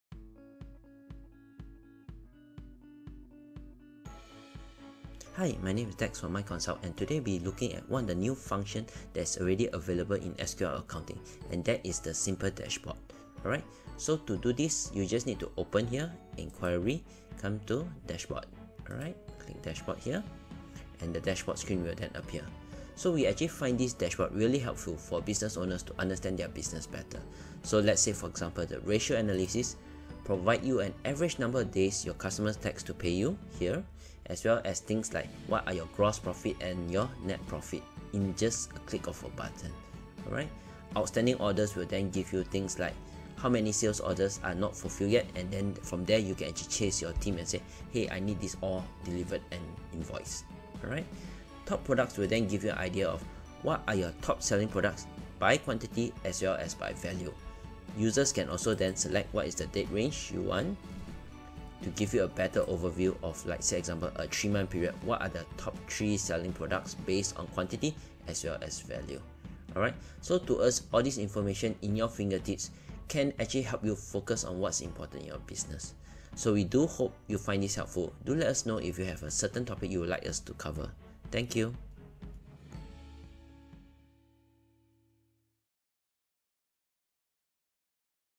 SQL Accounting Tutorial - Basic Financial Dashboard
Spending a lot of time trying to analyze your accounts?

Find out how easy it is to get this information in SQL Accounting now!